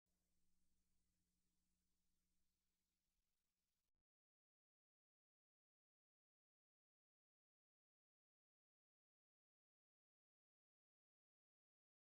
Tiny Amblimation logo
The Amblimation logo has gotten so tiny, it could be difficult for you to see Fievel.